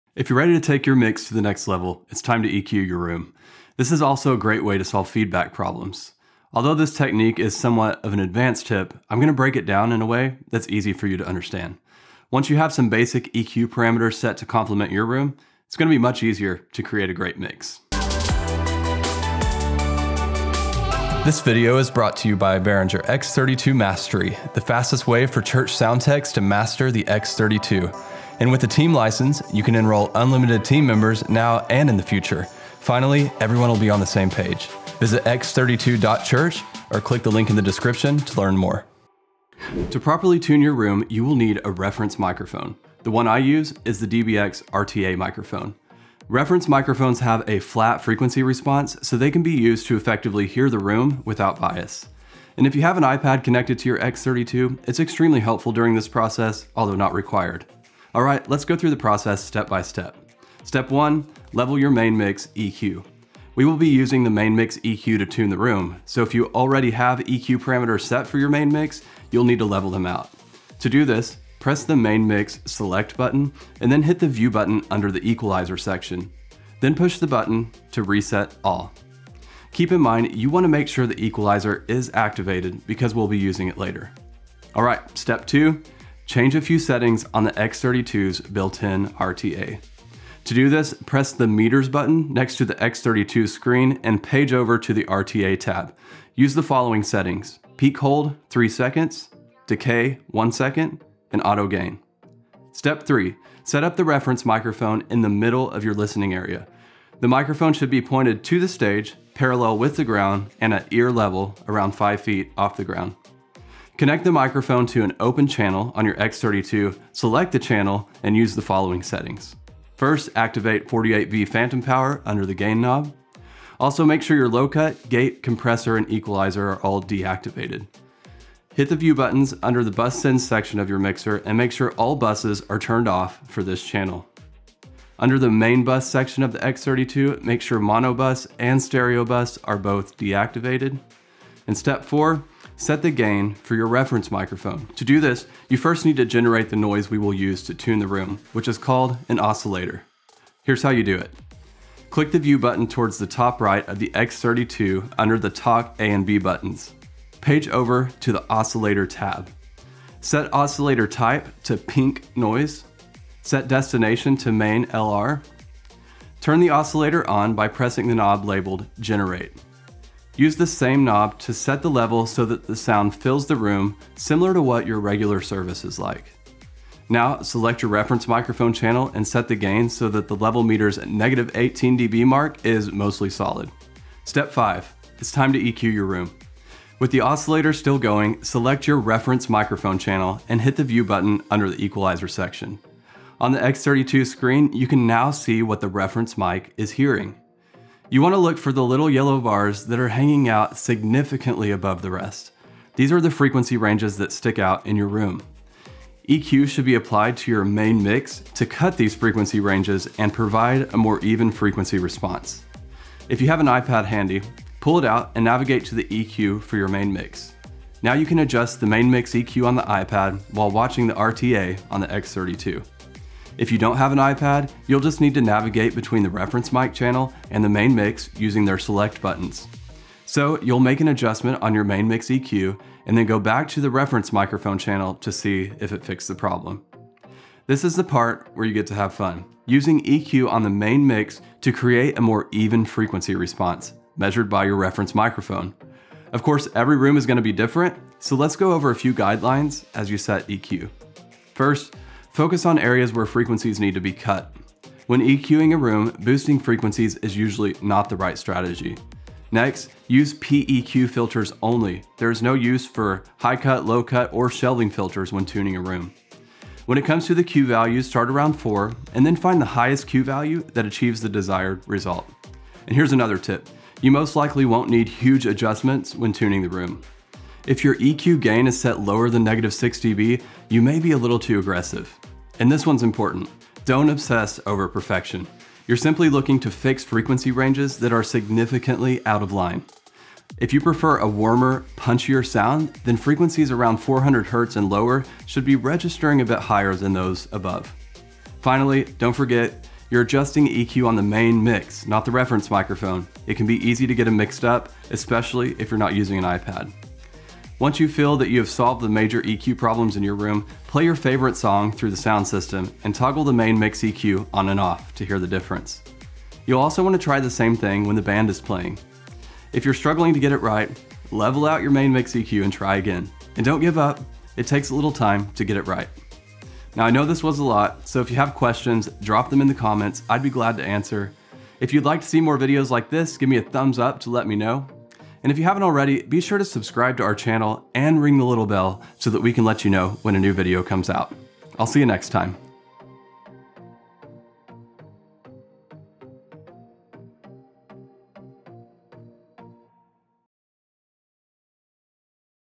How to EQ Your Room with the Behringer X32 (or Midas M32)
Enroll in X32 Mastery

If you are ready to take your mix to the next level, it is time to EQ your room. This is also a great way to solve feedback problems.

Although this technique is somewhat of an advanced tip, I am going to break it down in a way that is easy to understand.

Once you have some basic EQ parameters set to compliment your room, it is going to be much easier to create a great mix.

This video was made specifically for those using the behringer x32 or midas m32 digital mixer.